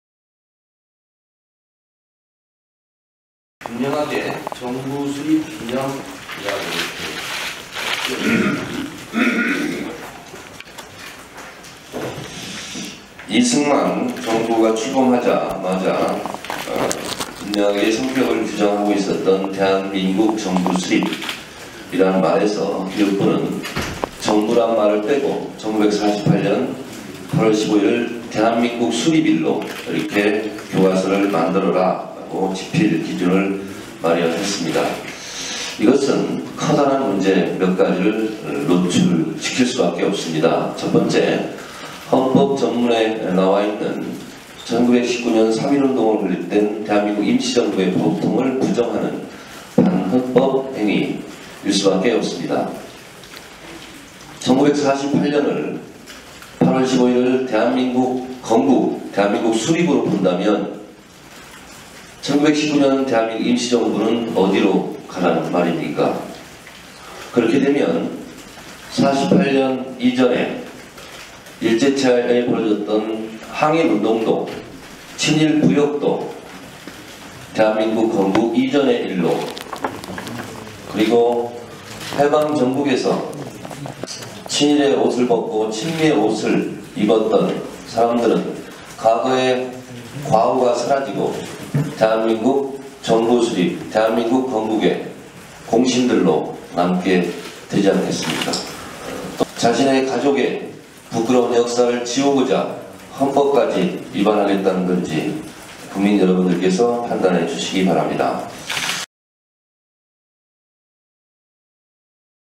이승만 정부도 인정한 정부수립
설명9일 오전 국회에서 열린 새정치민주연합 최고위원회의에서 정청래 최고위원은 대한민국정부수립 기념우표를 들고나와 이승만 정부도 정부수립으로 정확하게 인식하고 있었다며, 가족의 부끄러운 역사를 지우고자 헌법을 위반하겠다는 것은 아닌지 국민께서 판단해달라고 주문하고 있다.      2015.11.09.   촬영 이종현 기자